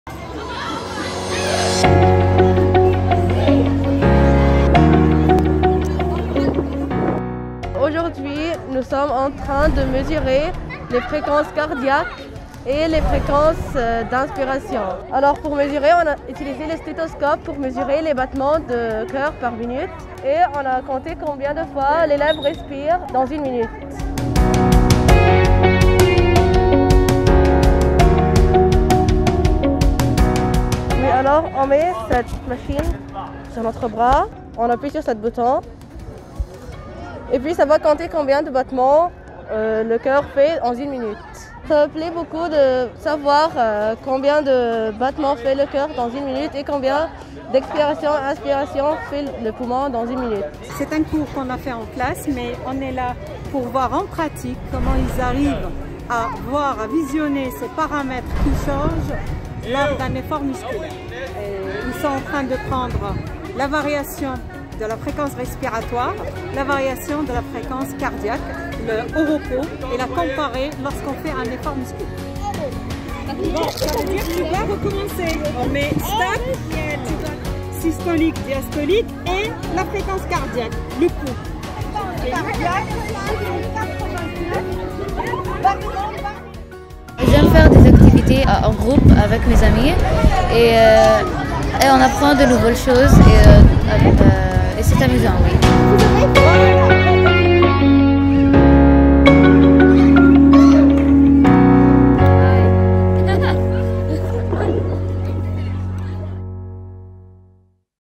experiment cour voltaire modification 1
This is a school experiment at Voltaire school in Cairo.

The footage was all shot using a samsung A51 mobile phone.

The editing was done using: Adobe premier, photoshop and illustrator